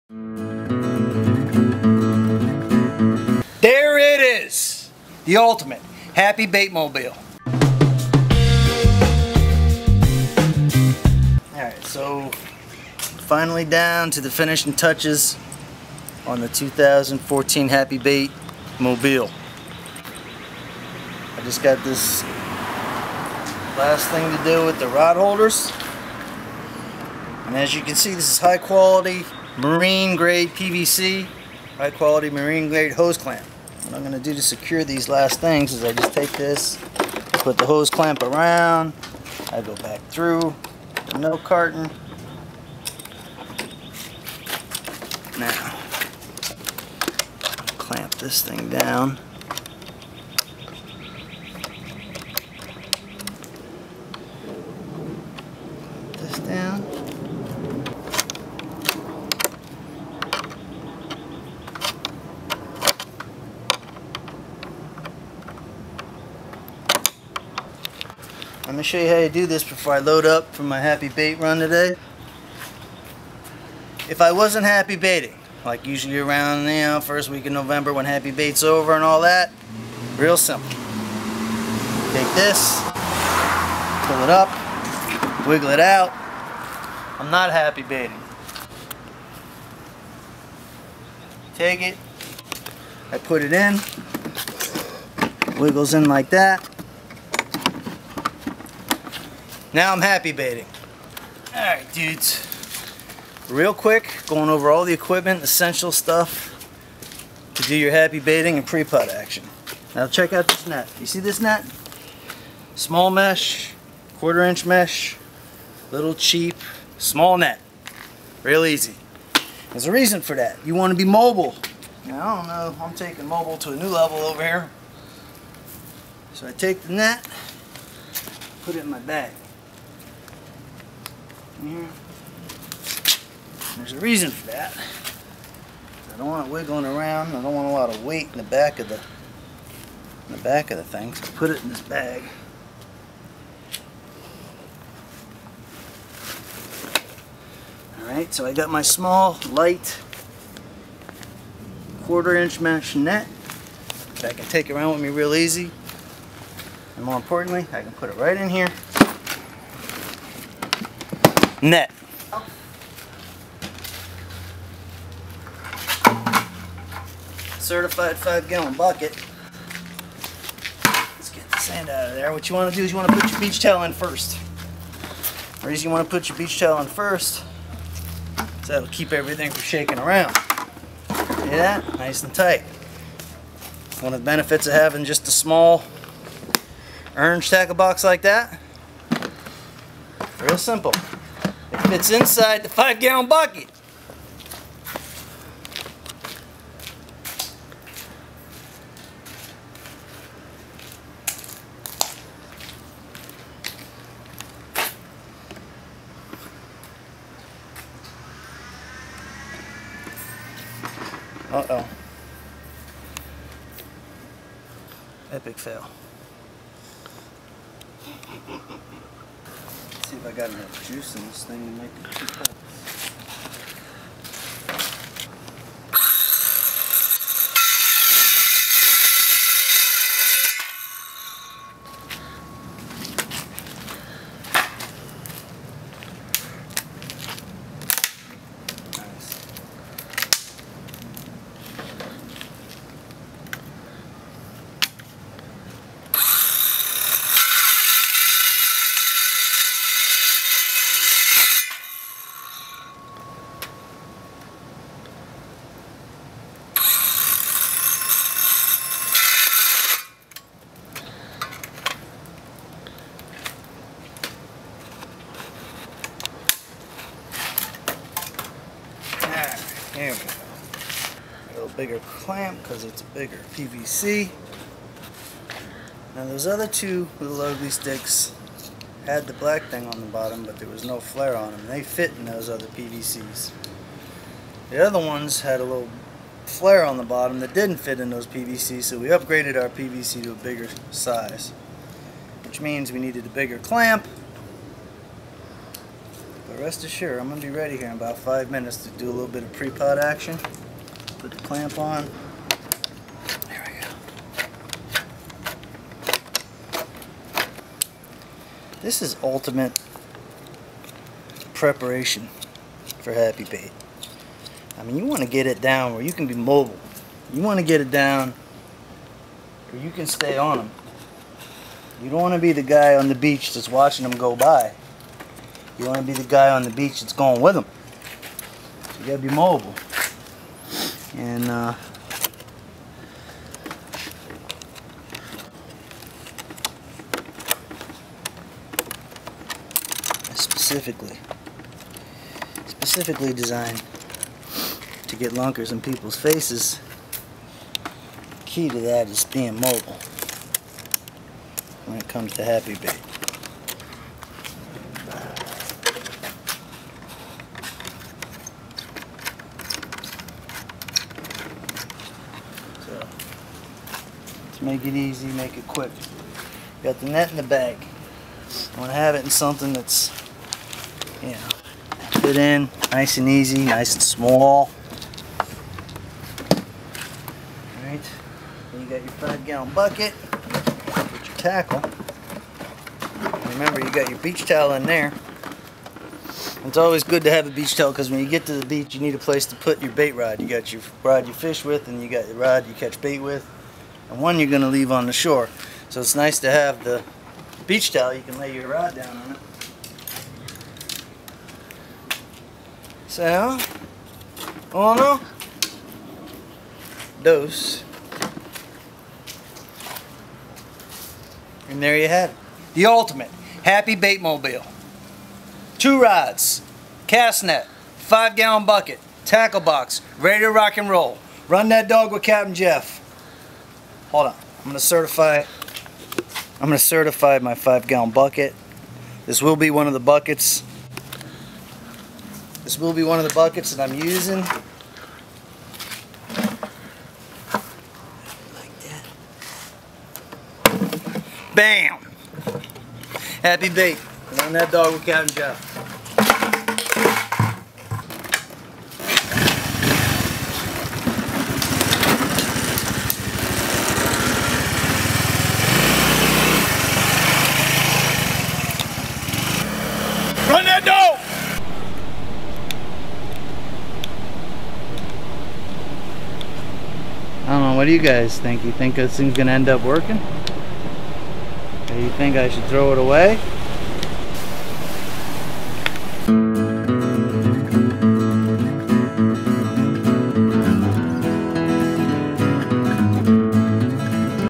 Fishing Scooter Ultimate Happy Bait
Custom Korean Scooter modified for Extreme Sportfishing.

During Happy Bait, its all about being mobile. The pods are moving south , and sometimes they are moving fast, and you can easily cover a mile or more trying to stand on a pod. So this scooter was modified for Happy Bait. The pro mod added marine grade components for the ultimate happy baiting experience. The net fits under the seat, and the 5 gallon bucket fits on the marine milk crate.